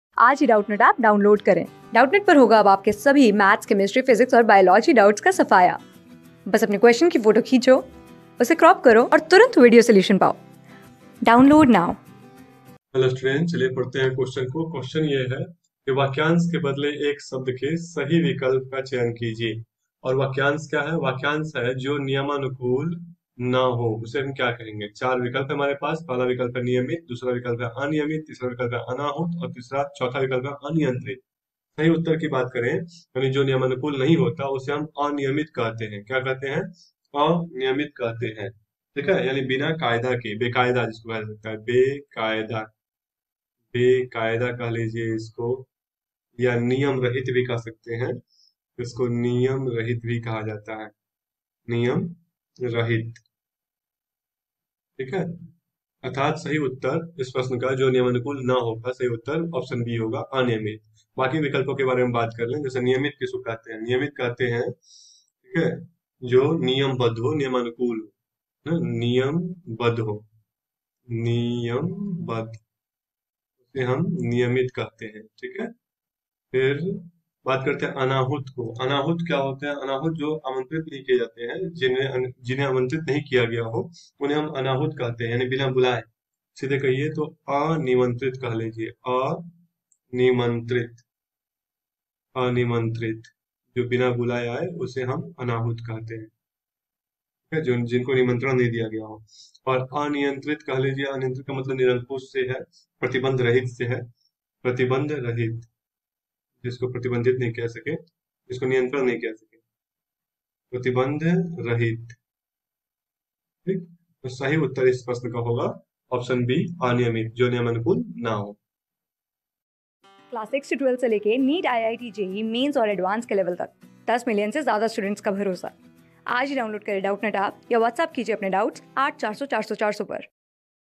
निदेश (प्र.सं. 31-40) दिए गए वाक्यांश के बदले एक शब्द के सही विकल्प का चयन कीजिए। जो नियमानुकूल ...
निदेश (प्र.सं. 31-40) दिए गए वाक्यांश के बदले एक शब्द के सही विकल्प का चयन कीजिए। जो नियमानुकूल न हो।
Class: 14
Subject: HINDI
Chapter: वाक्यांश के लिए एक शब्द
Board:GOVT

👉 Have Any Query? Ask Us.